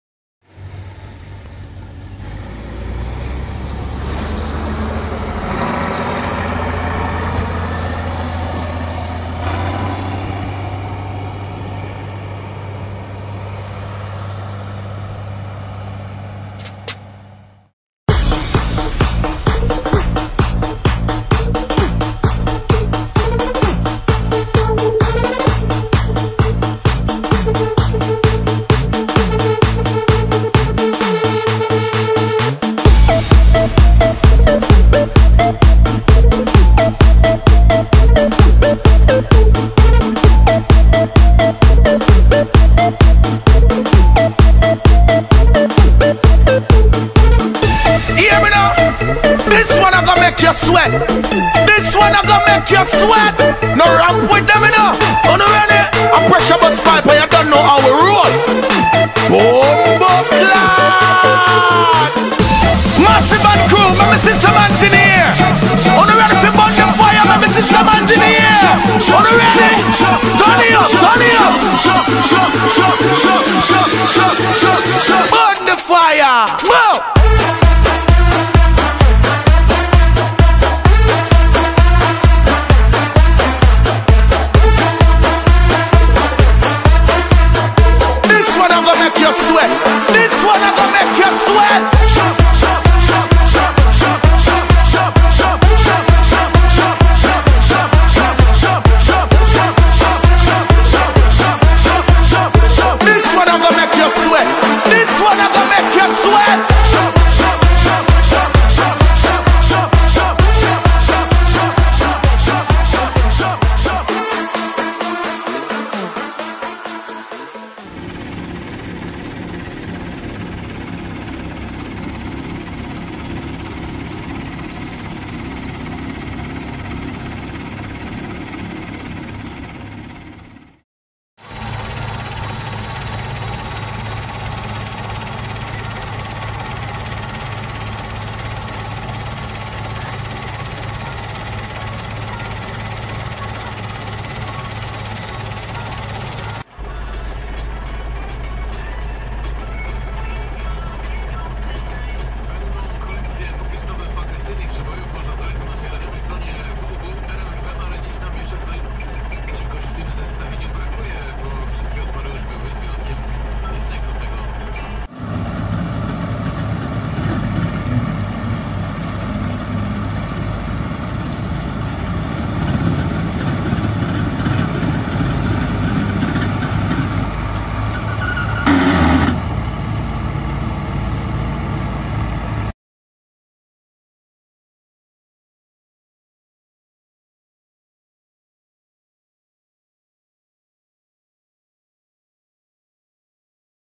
☆ Żniwa 2017 ☆ Bizon ZO56 ☆ Ursus 912 ☆ Koszenie Żyta ☆
Jakoś da się oglądać
Trochę Krótki ale to nic

Maszyny:
Bizon Z056
Ursus 912